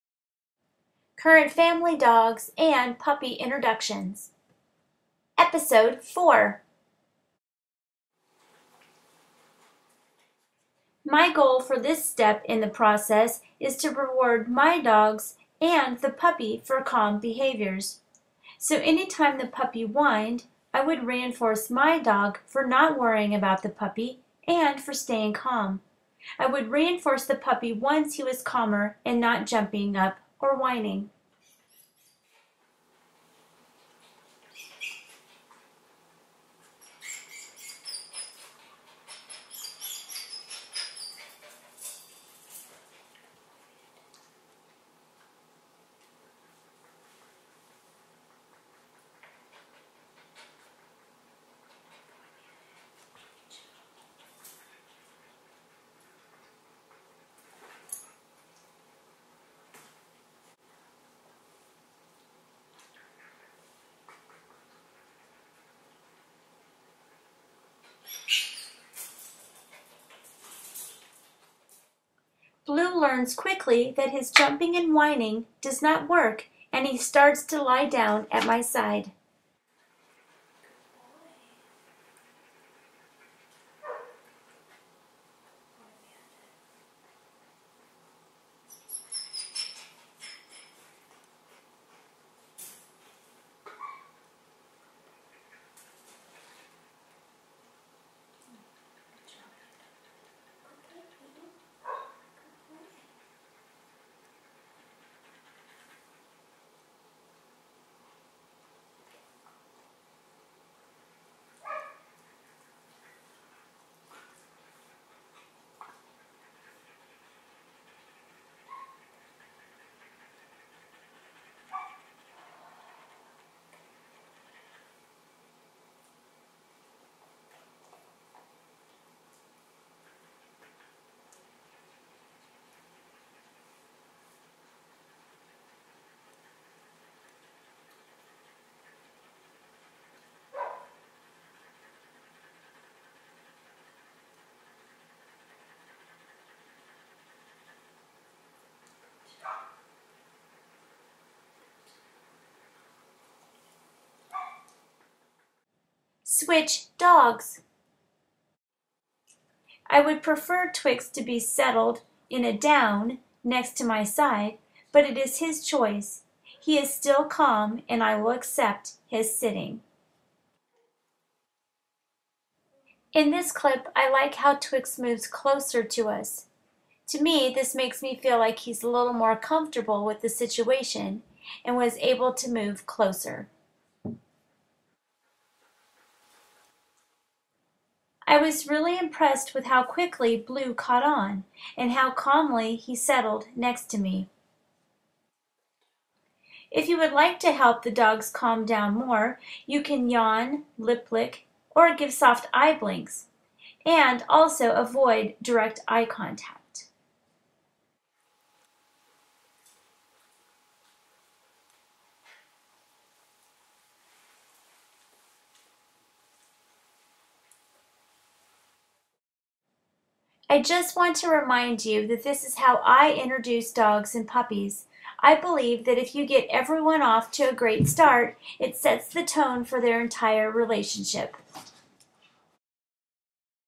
Family Dog's & Puppy Introductions - Episode 4
This is a 7 part series on how I introduced our Labor Day House Guest Blue (a 11 week old Border Collie) to my current family dog's (Isabelle, Bandit & Twix).

The introductions took a few hours, but it was worth it as we had an amazing weekend together.

There are many ways to train dogs to get along, but I happen to think that my way is the best.  :)

I reward calm behavior, introduce slowly, positively interrupt unwanted behaviors, and reward all behaviors that I like and want the dogs to repeat.  It really is simple and does not take much time at all.  It is sort of like teaching.  If you start the year off right and train the students to do the procedures and teach them how things are done, then the entire school year runs smooth.  Same with the dogs, if I teach them how I want them to behave by reinforcing good behaviors that I like, the inter-relationship between all the dogs is a positive one and everything runs smooth.  :)

Being a full time teacher, I like to relate a lot of what I do with dogs to humans and visa versa.  I am not saying that dogs are like humans, but when training and getting behaviors to be repeated, dogs and humans are very similar.  :)

Pam, Isabelle, Bandit, Twix & Blue